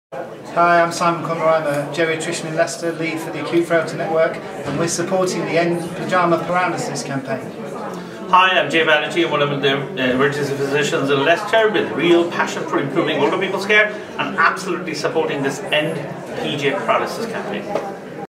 endPJparalysis supported by Simon Conroy & Jay Banerjee
This is really taking off changing mindsets and getting fabulous support from Simon Conroy & Jay Banerjee from University Hospitals Leicester